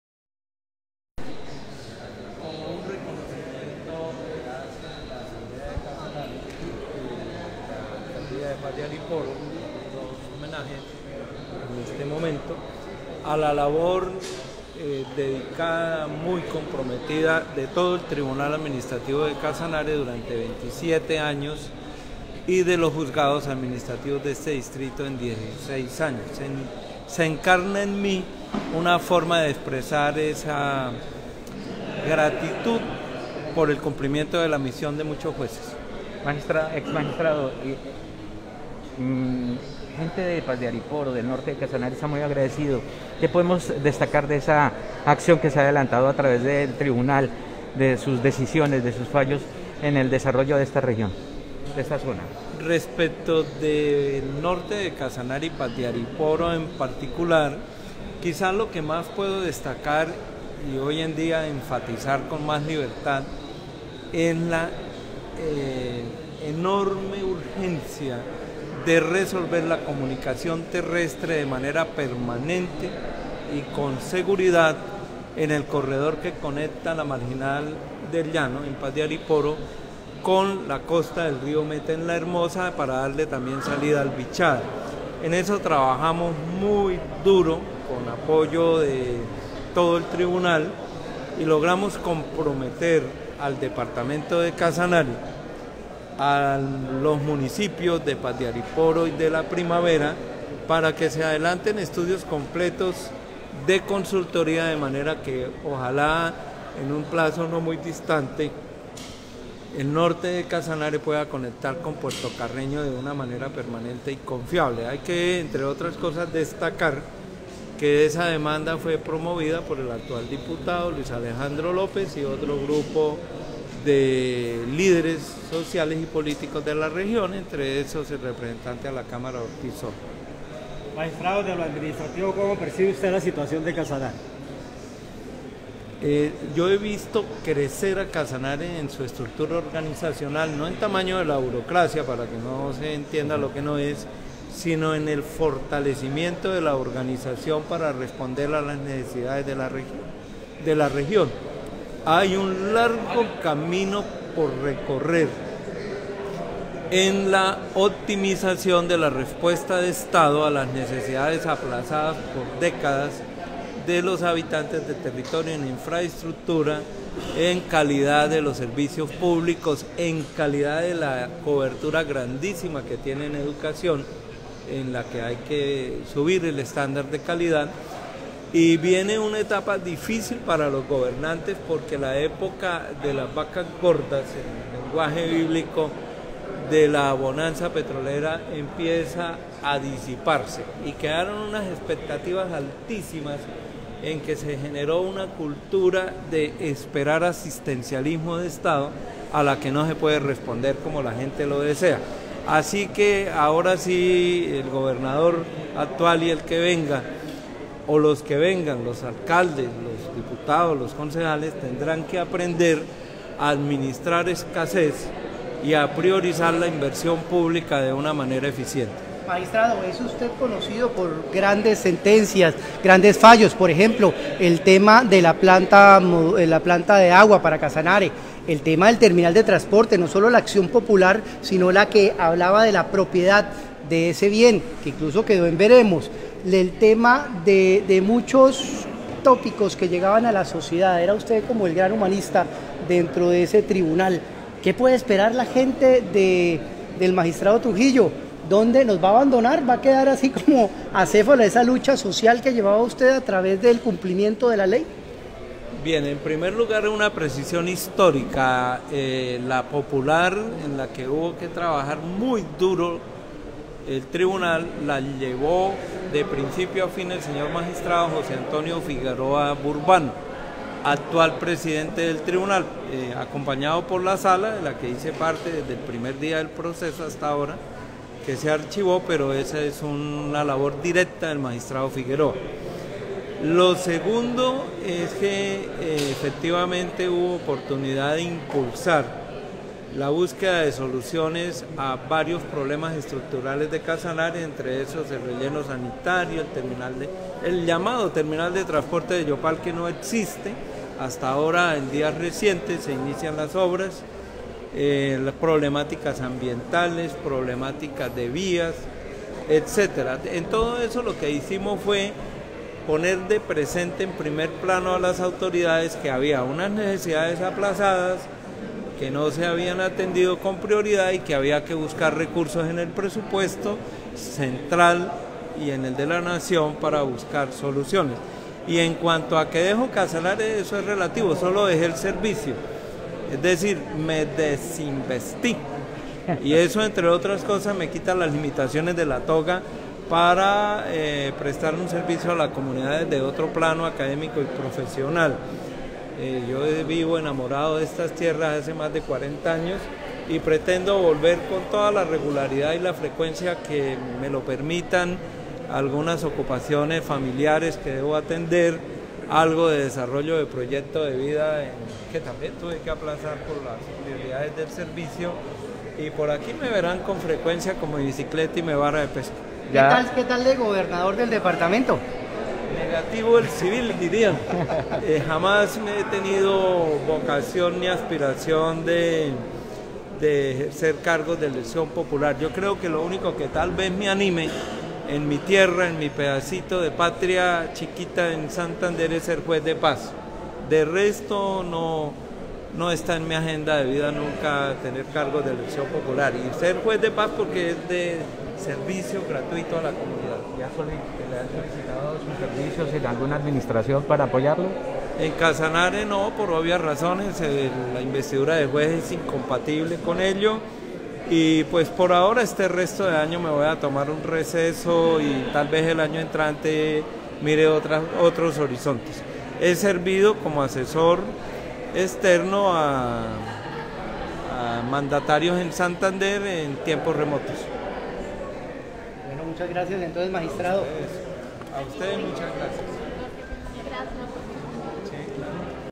Magistrado Néstor Trujillo recibió condecoración por su labor en Tribunal Administrativo de Casanare
Magistrado Néstor Trujillo recibió condecoración por su labor en el Tribunal Administrativo de Casanare